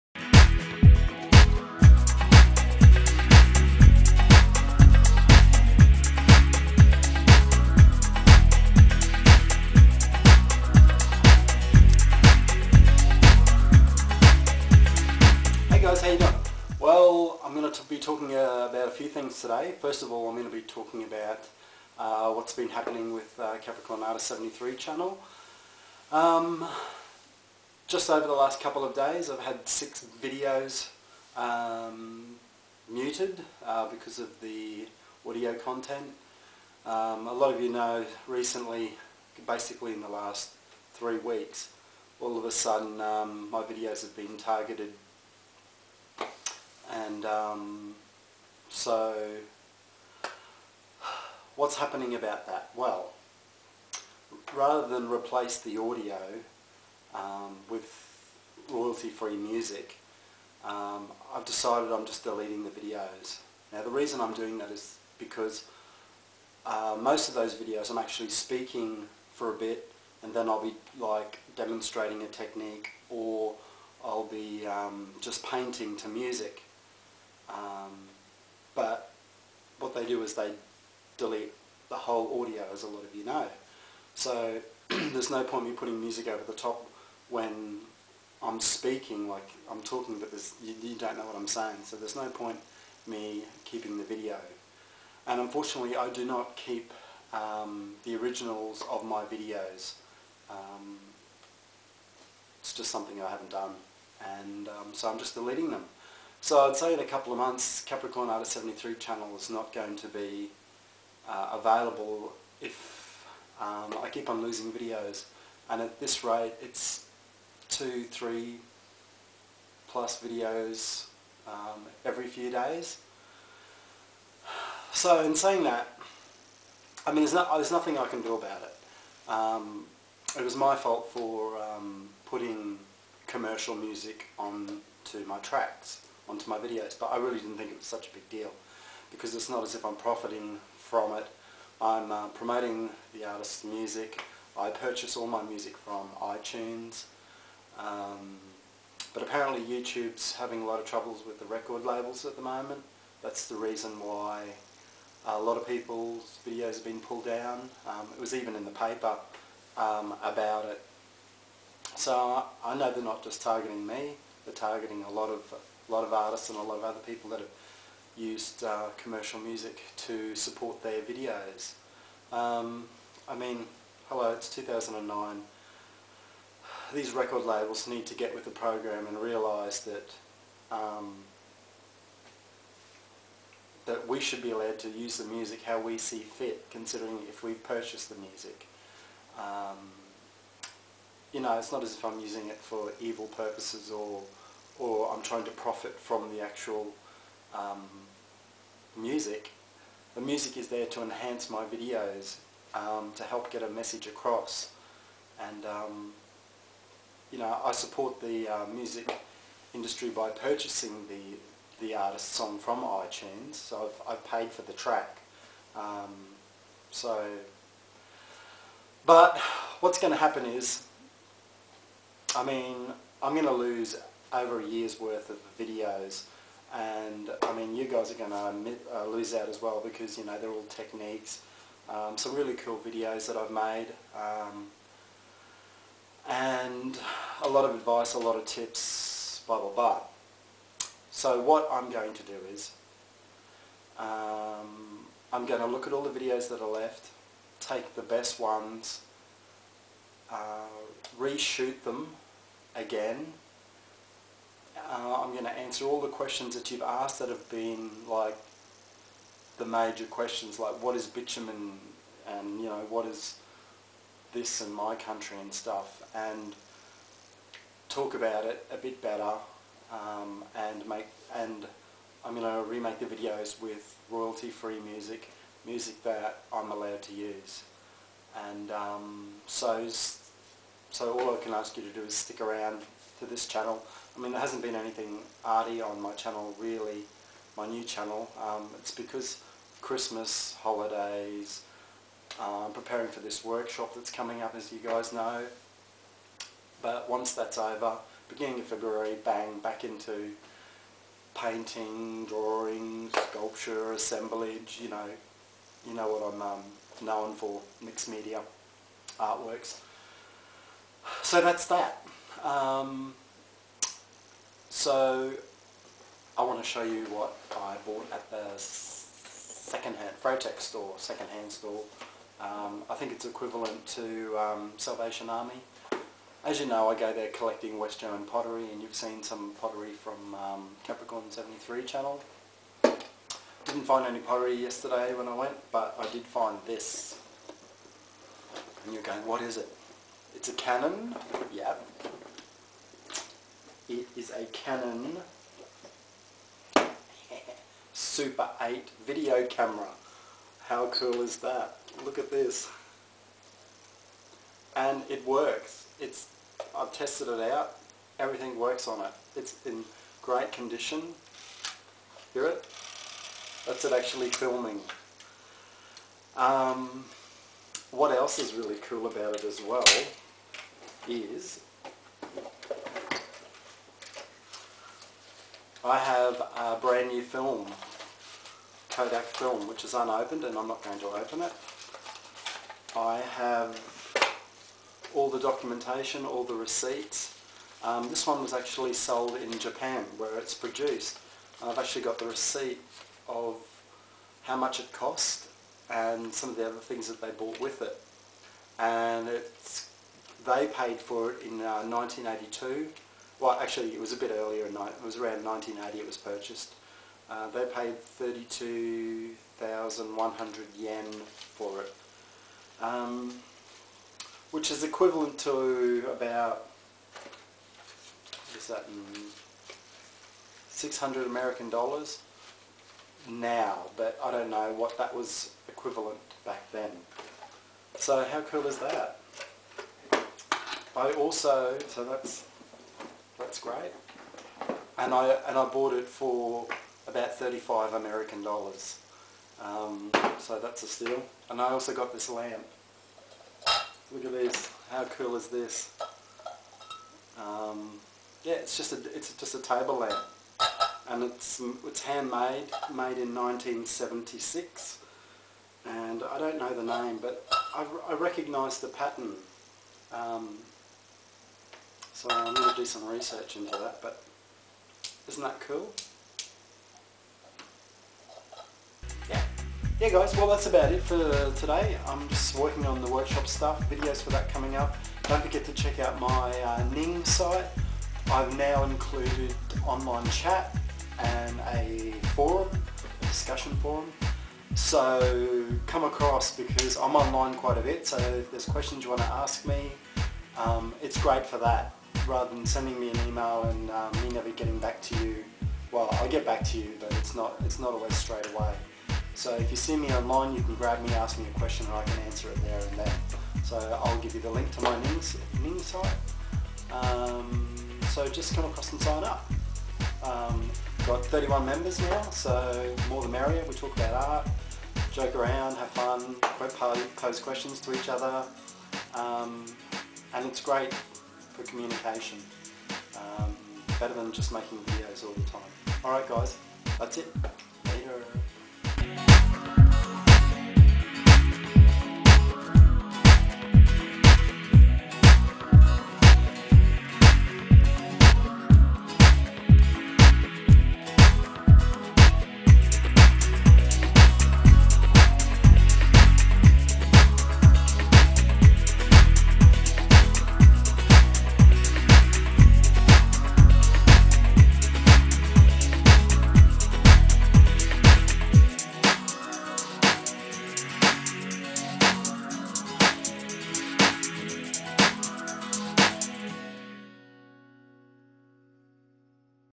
21st January update
Talking about my channel capricornartist73 and update.

You can blog, chat with me live, add your own artwork, your own blogs and discussions.